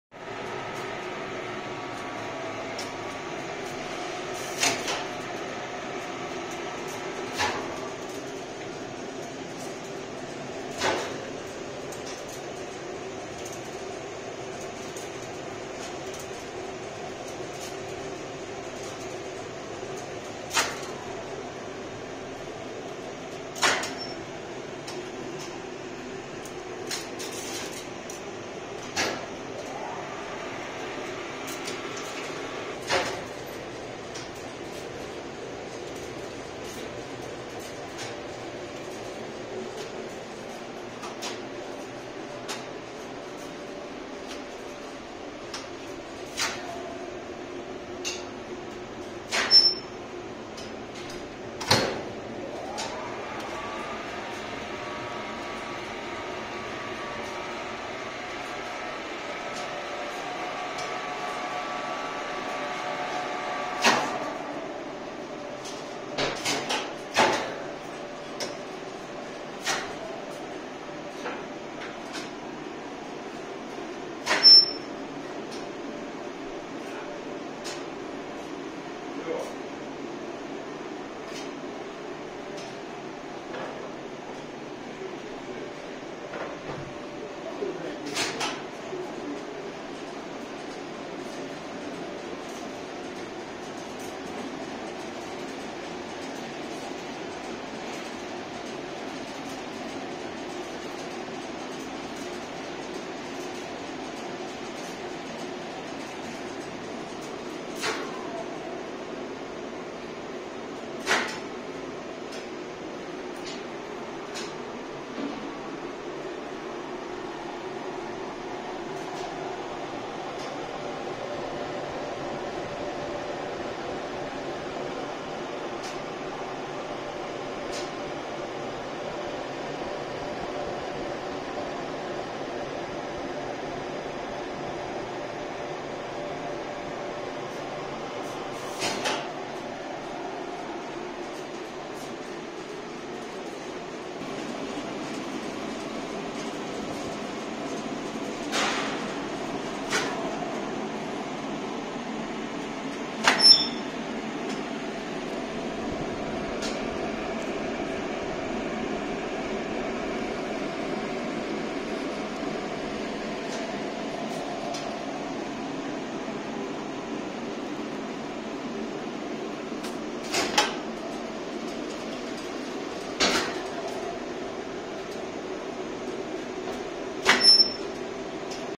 KONE Elevator SSP
KONE Elevator. SSP - relay elevator.
ADB Door Drive.
Relay station with control of three elevators in a group.
Double speed winch. 1987 year of release.